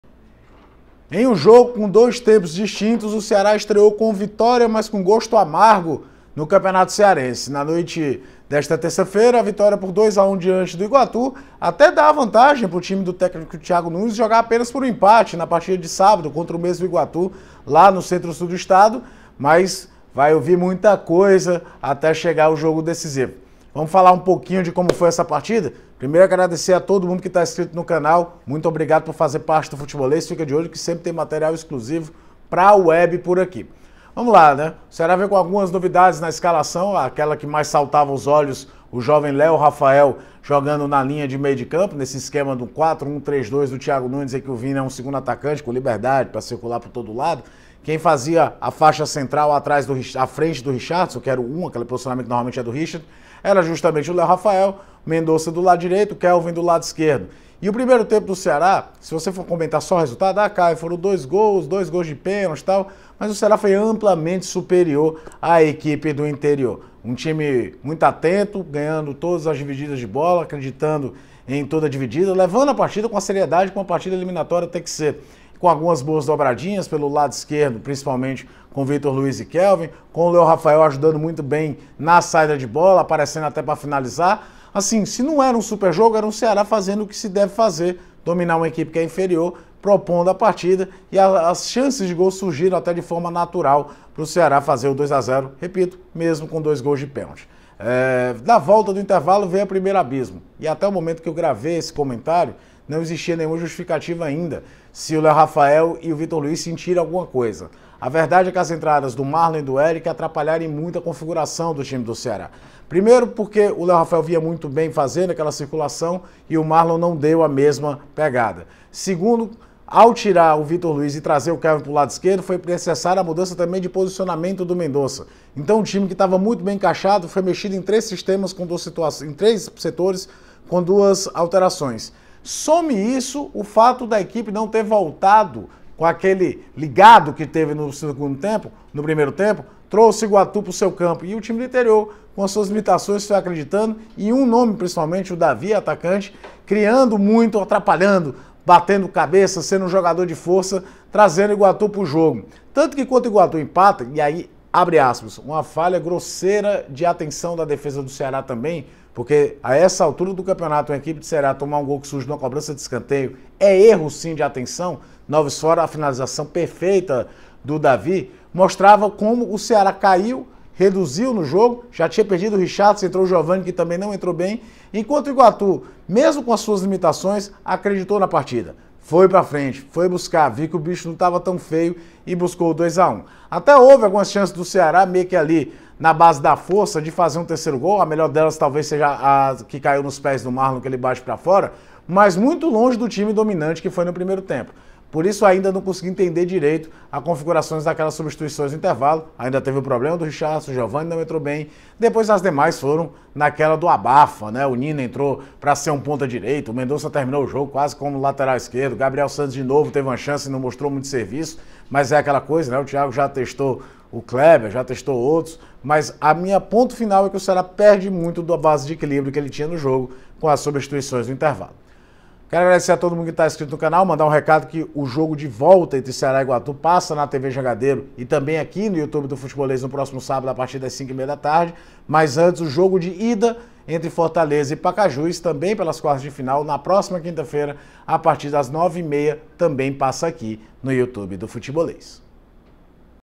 CEARÁ 2 X 1 IGUATU - VOVÔ VENCE, MAS VÊ IGUATU REAGIR NO SEGUNDO TEMPO
Ceará domina  no primeiro tempo, mas vê Iguatu crescer na etapa final. Confira a análise de Caio Costa.